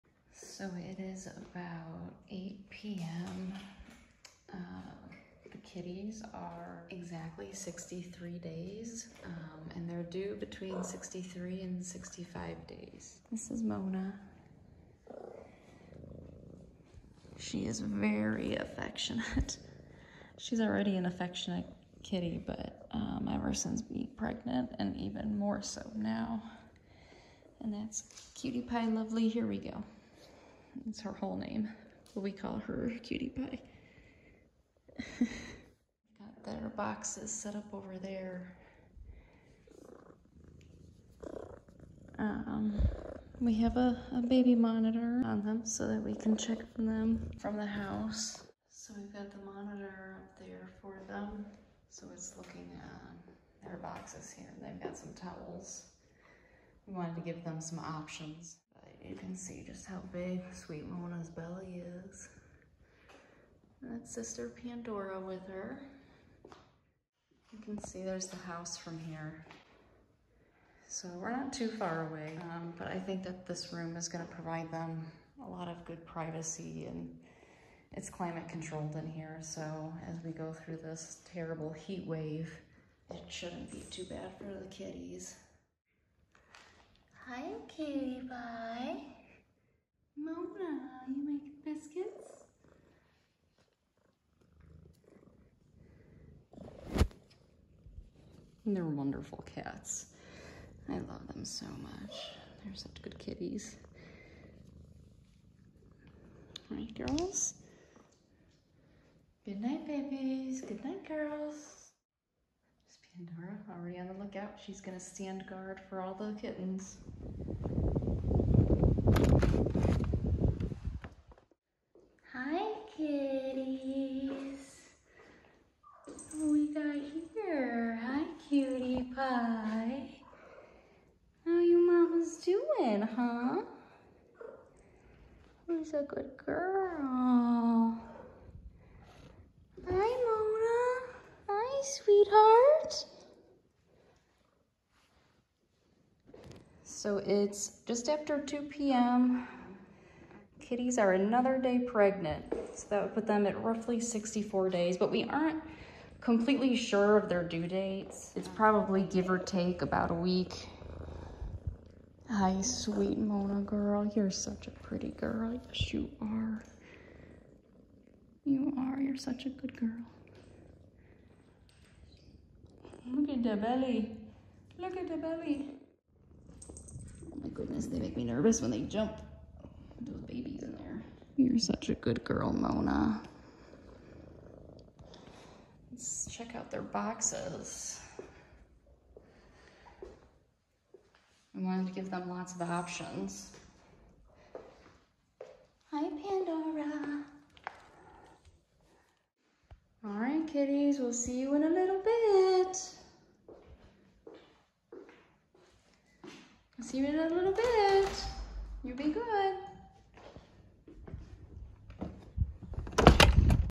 Checking in on our Expecting Mama Farm Cats
Two of our farm cats are expecting kittens. They are due any day now, so we have them set up with their boxes in a private place. We chose the “grandma shed” - an unfinished in-law apartment - for the cats to deliver their kittens in peace without our dogs interrupting or becoming a distraction. The last thing we want to do is stress them out right now.

We plan to raise these kittens in a similar fashion to our puppies, as cats also make fantastic Emotional Support Animals. Mona serves as my ‘backup’ ESA to Daisy, and it is actually really nice to have both a dog and a cat for that purpose. Sometimes I need Mona’s loving chill over Daisy’s loving energy.

As the litters are born, we will livestream the kittens just like we stream our puppies. With Myra’s puppies finally all heading home, the stream will move from our adult dogs to the kittens.

We are excited to share this journey with our community and look forward to updating you all with great news!